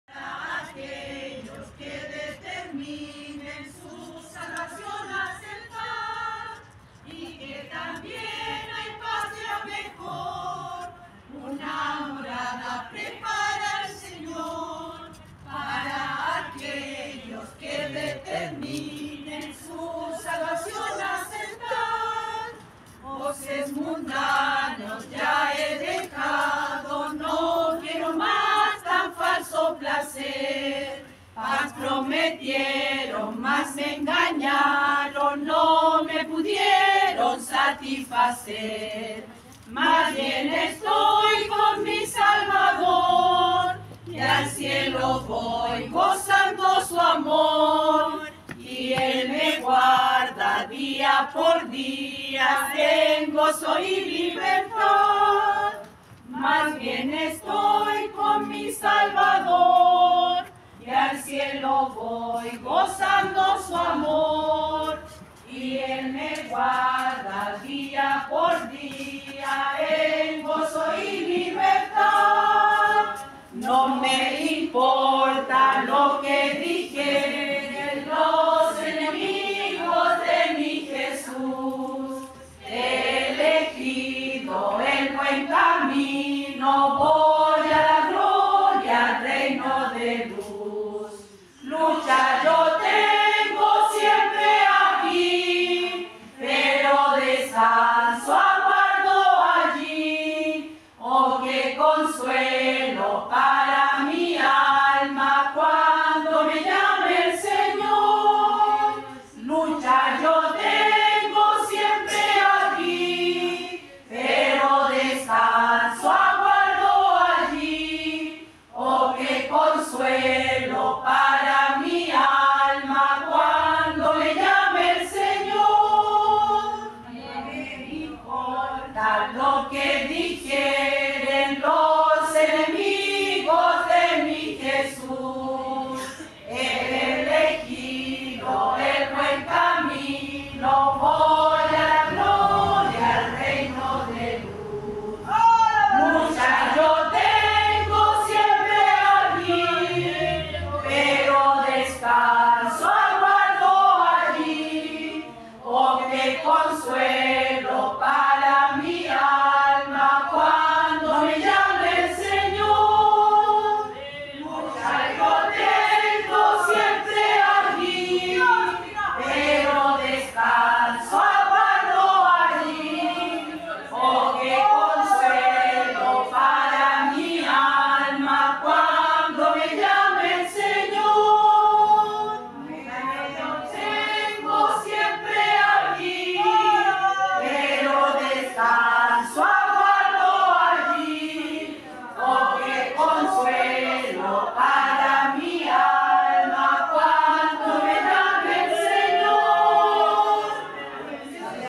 Hnas Dorcas Voluntarias IEP Sargento Aldea Visitando el Local Yarur
Hermanas Dorcas Voluntarias, Iglesia Evangélica Pentecostal Sargento Aldea, Chile, visitando el Local Yarur, el viernes 18 de Agosto de 2023. Nuestras hermanas Dorcas cada viernes predican de 14:30 a 16:00 Hrs. visitando los locales de la iglesia. Dios bendiga a nuestras Hnas.